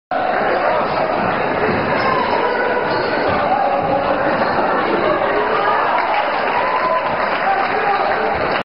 GC: Louis Young Basket
GC: Louis Young Basket - High School Boys Basketball: Good Counsel vs. St. John's 01/03/2009